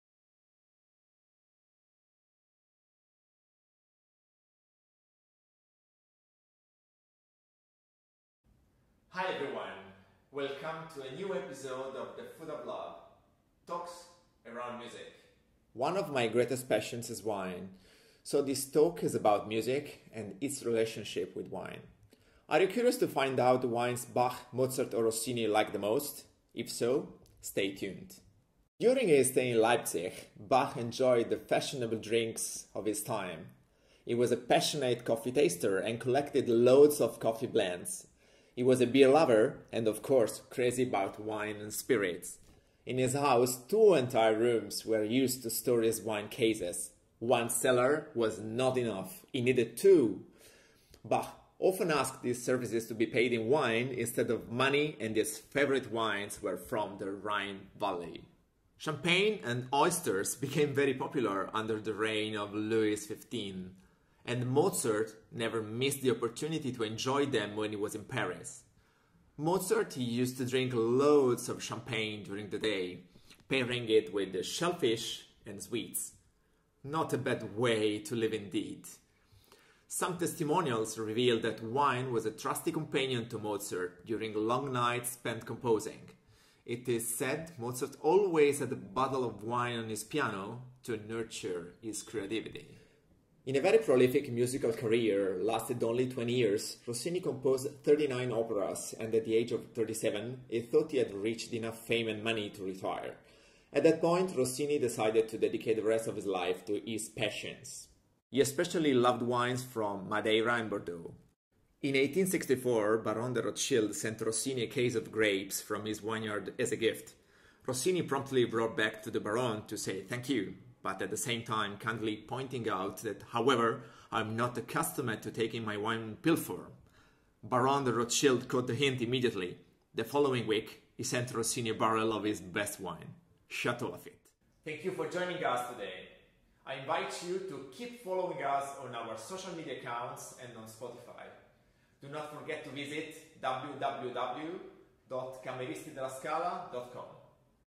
The Food of Love, wine and composers
Has anyone ever told you what Bach, Mozart and Rossini liked to drink the most?
The Food of Love, talks around music by Roberto Nigro and Cameristi della Scala